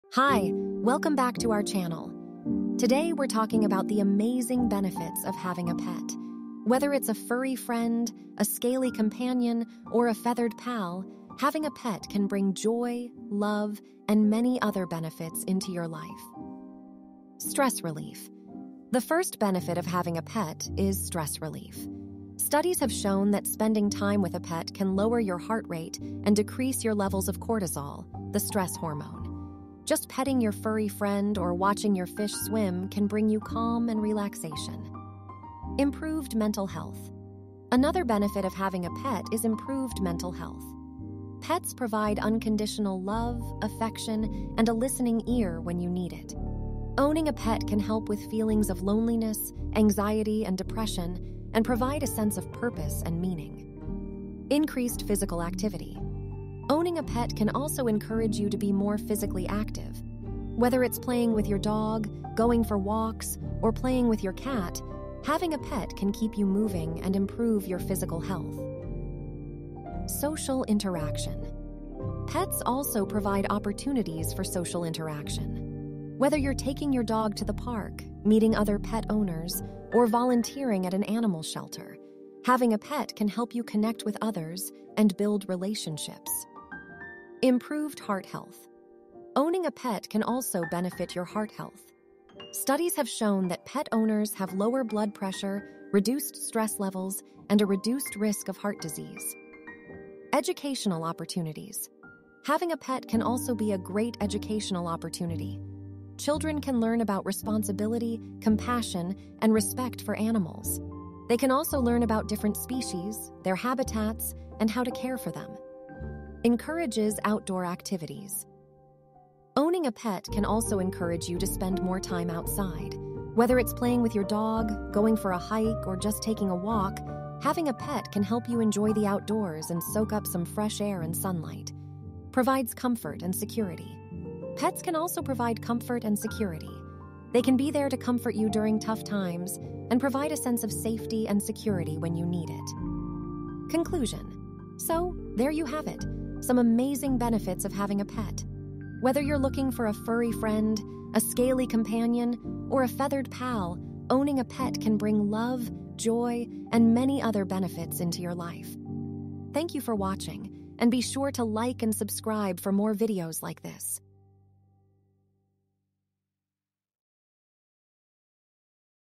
Pets - What Can They Do For You?
Many people choose to add a pet to their family because they believe that pets provide many benefits. Whether you're looking for mental health benefits, comfort and security, or just want a friendly animal to share your home with, there are many reasons to consider getting a pet. In this video, we'll explore some of the many benefits of having a pet.

Are you fascinated by the world's wonderful animals? We'll share amazing facts, inspiring stories, and detailed information about the world's greatest animals.